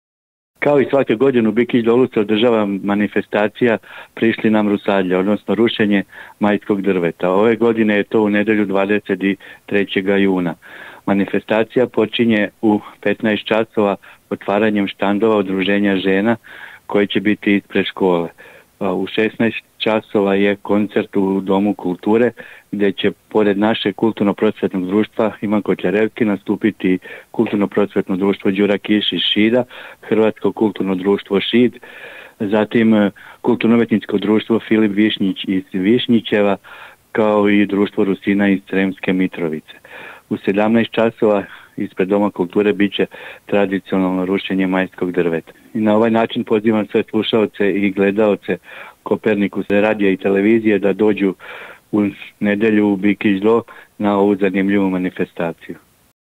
FONO: Dejan Bobalj - Najava "Rušenja majskog drveta"
U nedelju 23. juna u organizaciji Kulturno prosvetnog društva „Ivan Kotljarevski”, udruženja žena „Bikićanke” i mesne zajednice Bikić Do biće održana manifestacija „prišli nam rusadlja 2024.”. Manifestacija počinje u 15 časova, a svečano rušenje maja je u 17 časova. Više o samom događaju nam je rekao Dejan Bobalj, predsednik mesne zajednice Bikić do.